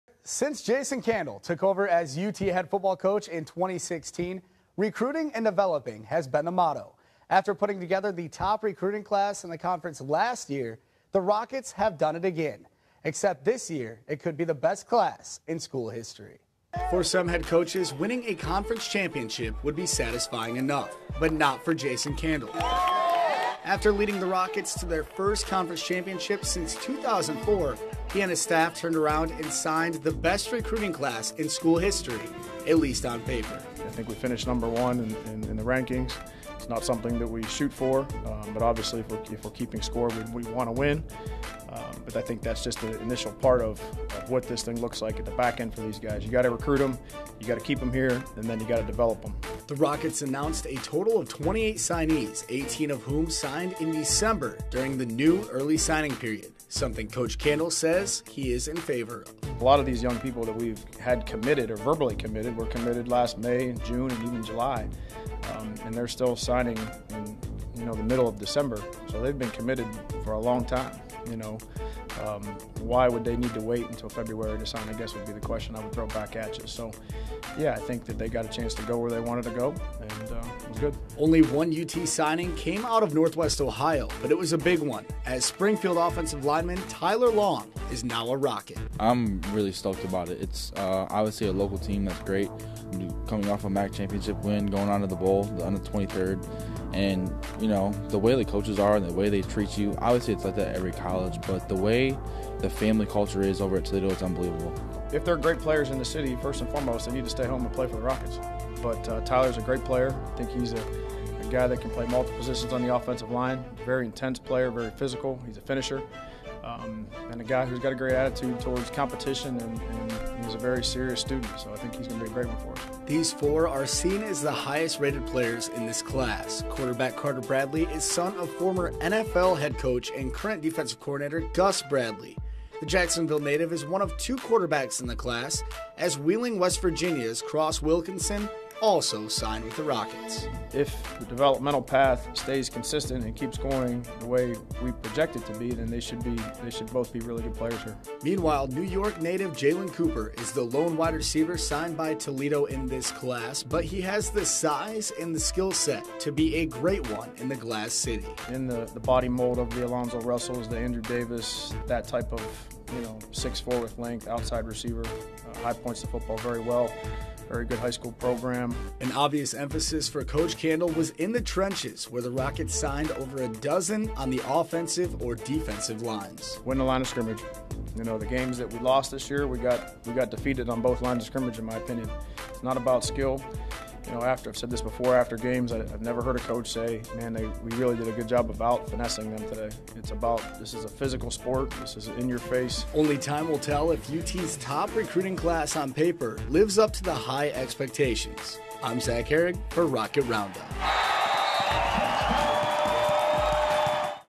Recapping Toledo Football's 2018 Recruiting Class
Jason Candle & his coaching staff have been busy. After coming off of the program's first conference championship since 2004, they signed an unbelievable recruiting class.